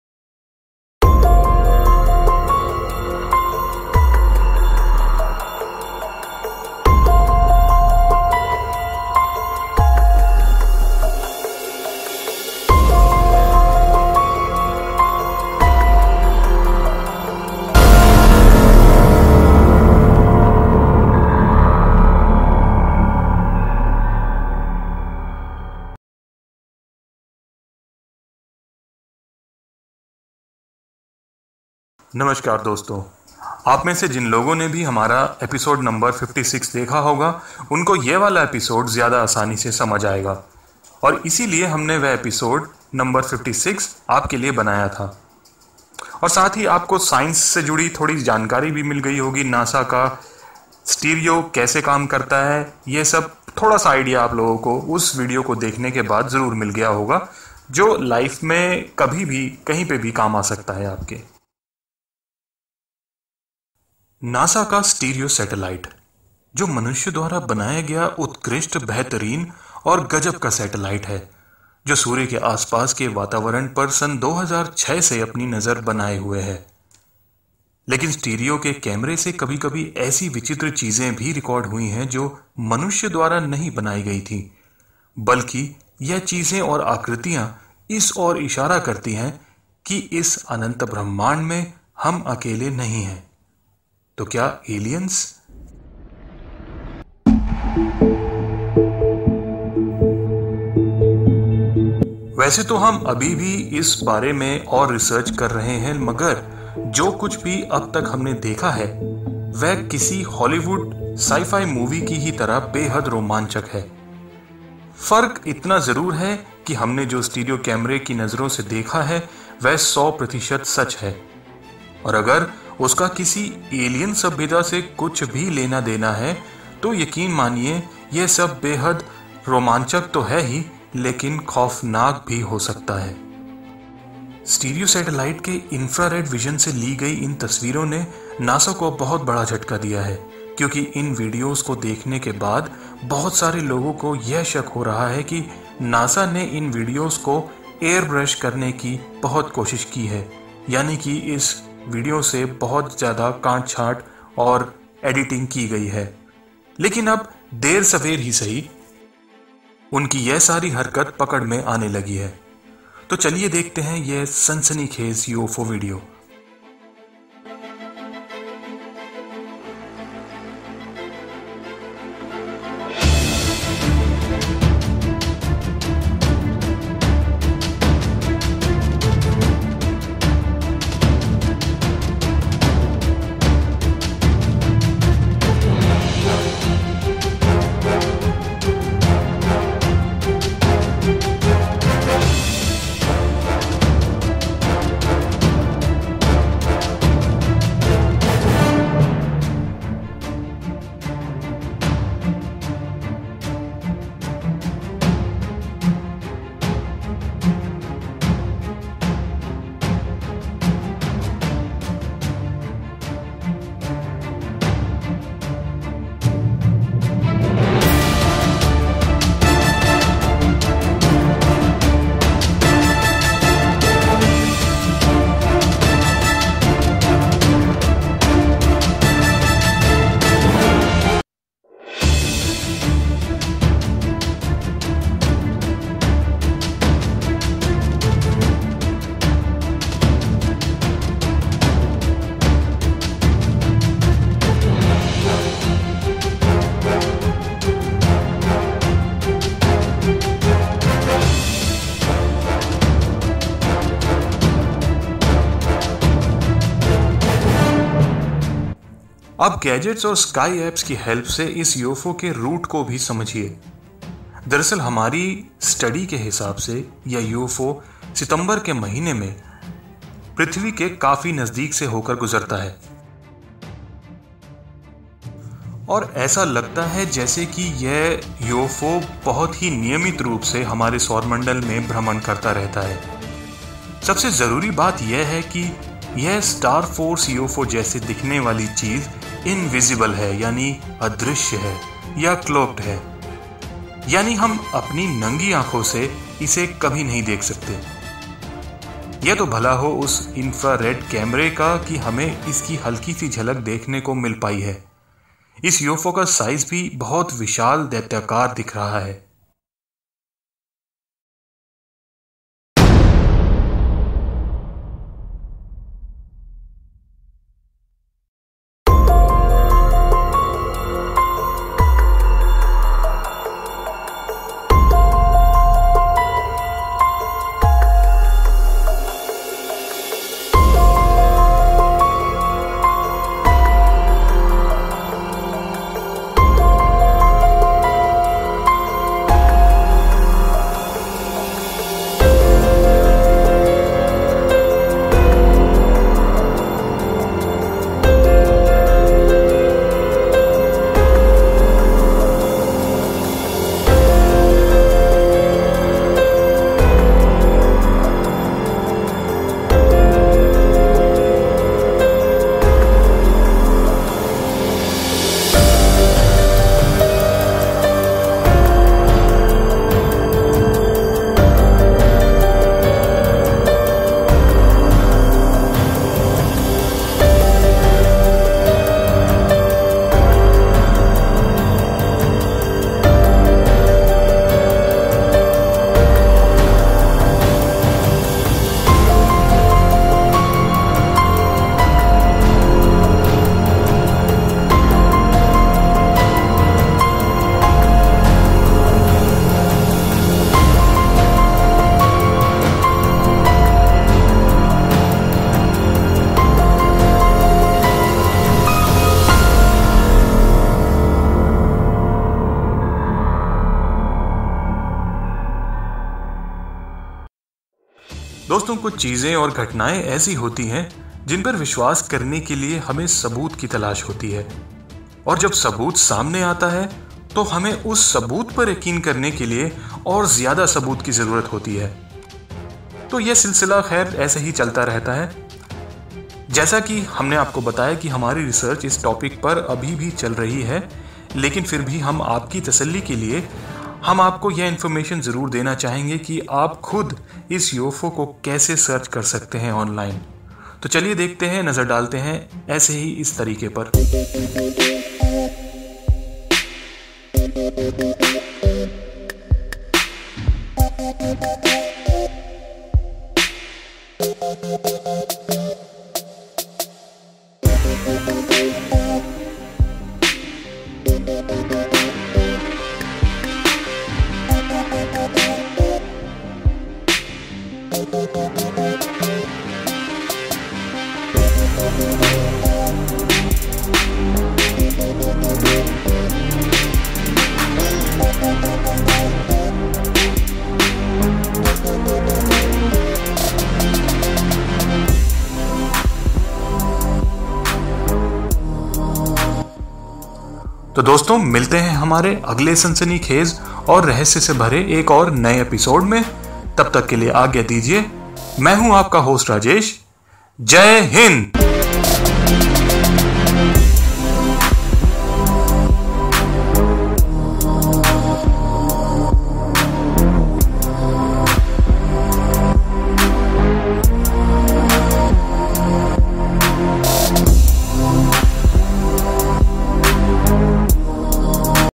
Infra-red Camera confirms Star-ship UFO flying near Sun Episode 57
STEREO (Solar Terrestrial Relations Observatory) is a solar observation mission. Two nearly identical spacecraft were launched in 2006 into orbits around the Sun that cause them to respectively pull farther ahead of and fall gradually behind the Earth.
Now, UFO hunters have found an amazing unknown star ship like object flying very near to our Sun. It was possible only because of NASA's STEREO satellite and its IR cam. Can you solve this?

Credit: NASA's STEREO website